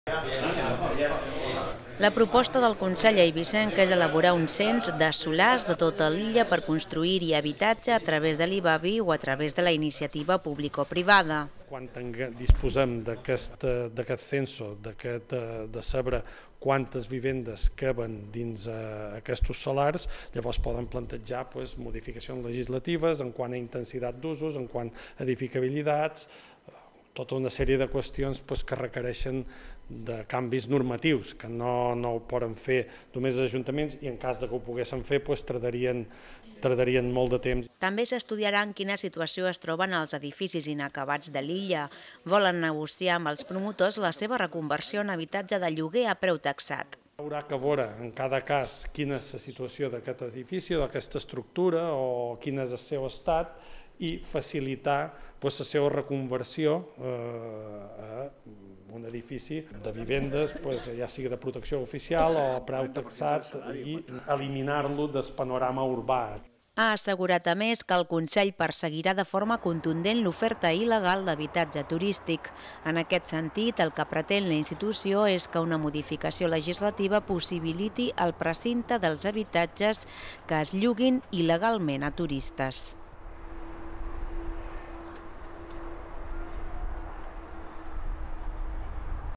El primer Consell d'Alcaldes de la legislatura decideix crear un cens de solars on es pugu
La institució vol impulsar canvis de la normativa urbanística per agilitzar la construcció de pisos de lloguer a preu taxat o HPO